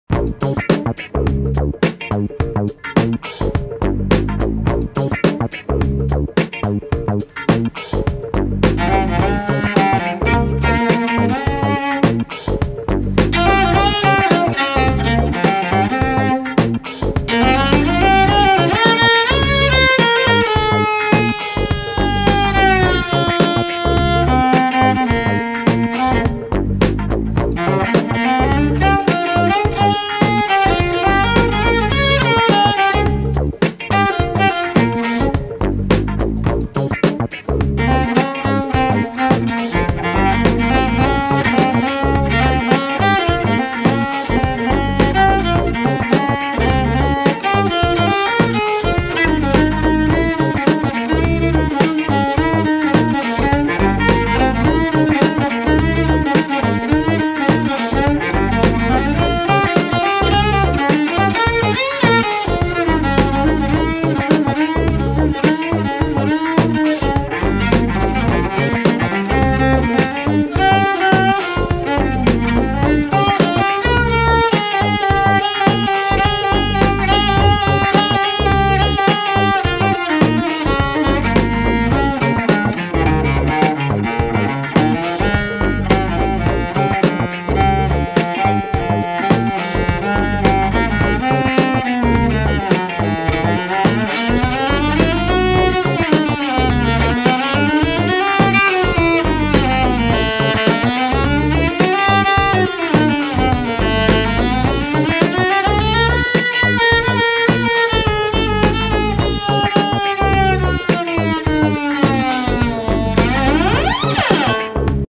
One chord improvisation (B flat) jazz cello джаз виолончель импровизация
Alexey Listochkin, cello. Видео и аудиозапись, сведение, свет, монтаж - Сергей Листочкин. Трек записан в квартире 253 :)